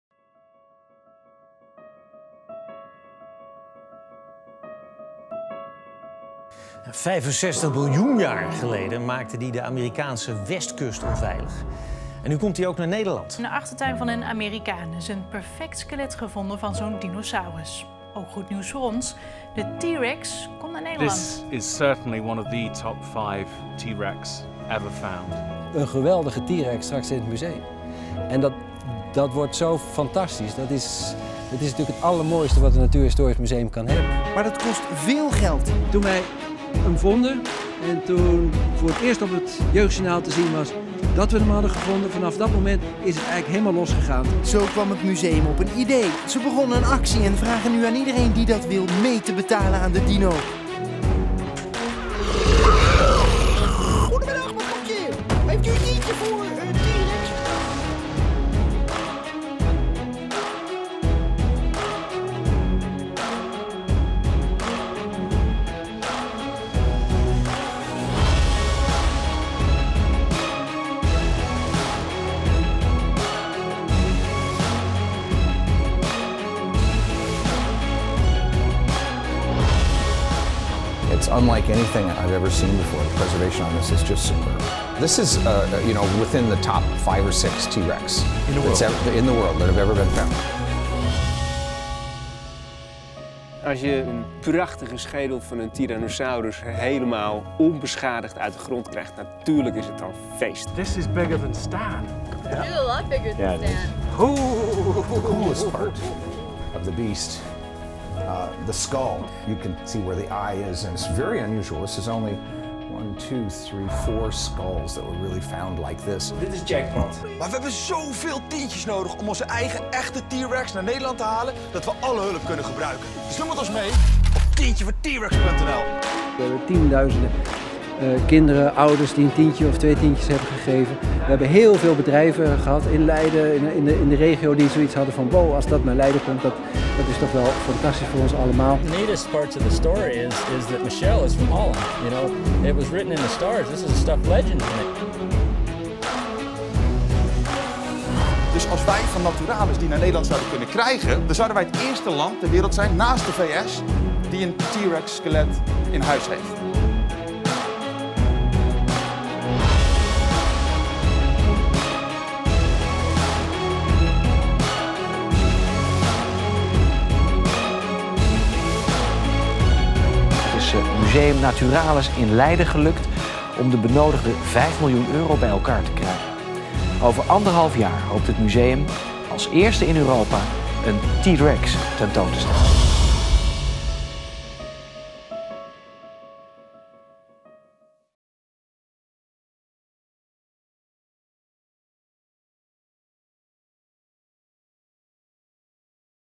De T. rex komt naar Nederland!
In september 2013 groeven paleontologen van Naturalis het skelet van Tyrannosaurus rex op. De vondst zorgde bij veel mensen voor betrokkenheid bij de wens van Naturalis om het skelet naar Nederland te halen. Dit is gelukt, want de T. rex komt naar Naturalis!

Wil je weten hoe dit precies gegaan is? Kijk dan deze video! We nemen je mee terug naar september 2013 waar het allemaal begon...